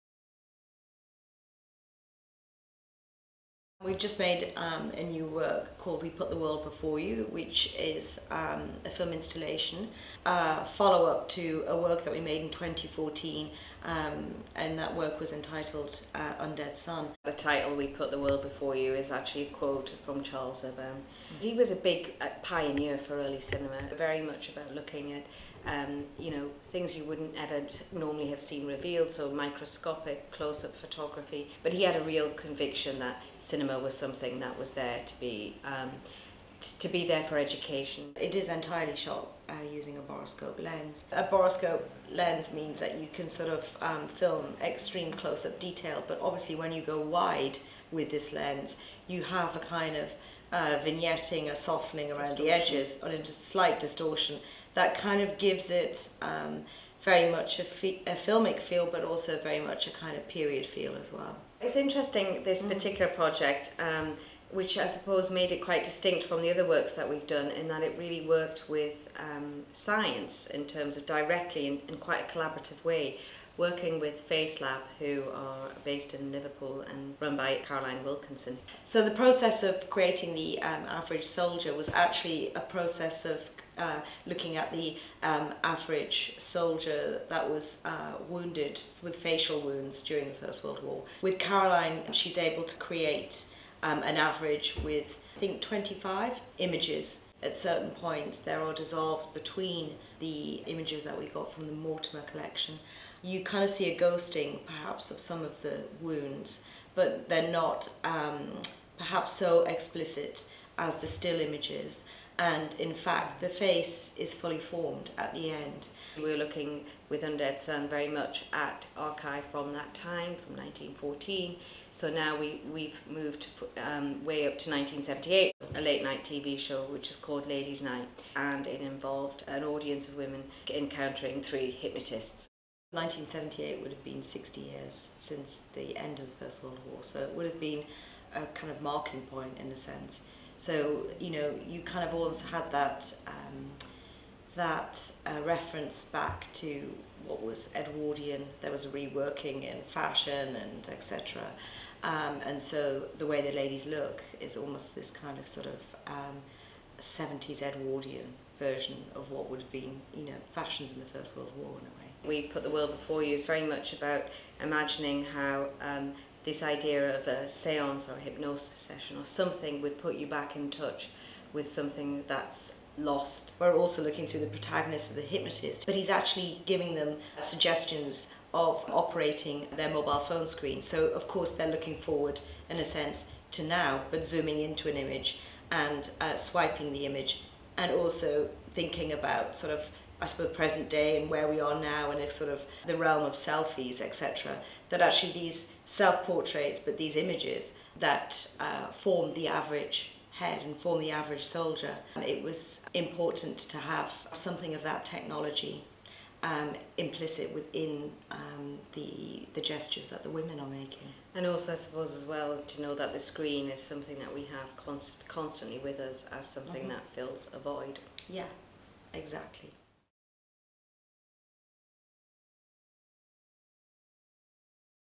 FVU FRAMES: Jane & Louise Wilson, 'We Put the World Before You', 2016
Combining sensitively-handled archive imagery of disfigured soldiers and newly constructed scenes alluding to seance and hypnosis, Jane & Louise Wilson's 'We Put the World Before You' draws together the many attempts at restoring physical and emotional voids left by war. This continuing pursuit has resulted in extraordinary developments in technology - a theme which continues to captivate the Wilsons. The resulting commission demonstrates how sophisticated technology, and perhaps more illusory techniques, might restore life and form to what seemed irrevocably destroyed. In this short FVU Frames interview, the artists discuss the ideas and techniques behind their work.